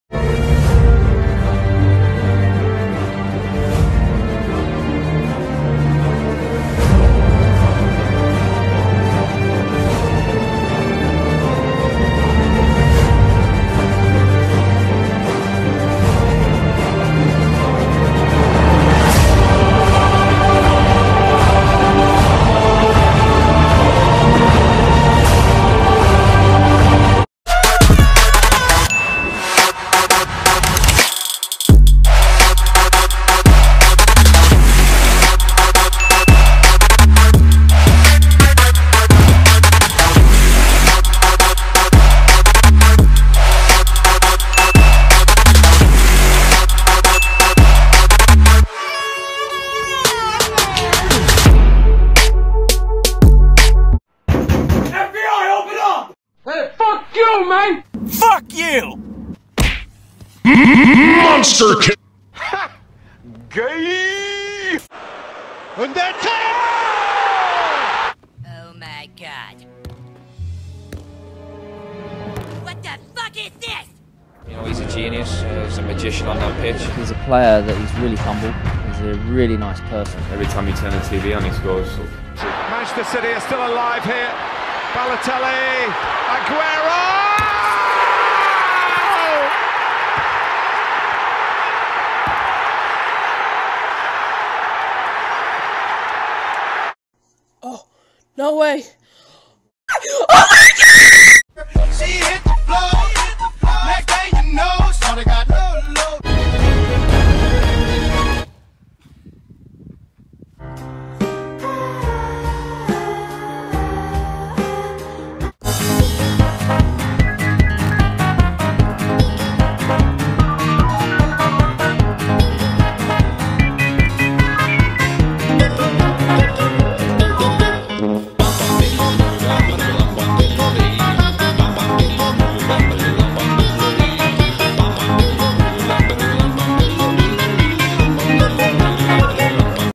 TORN CROSSBAR CHALLENGE & Epic Fails
Yes Guys! Another video is here ! Today its a Crossbar Challenge with torn balls and some pretty good fails.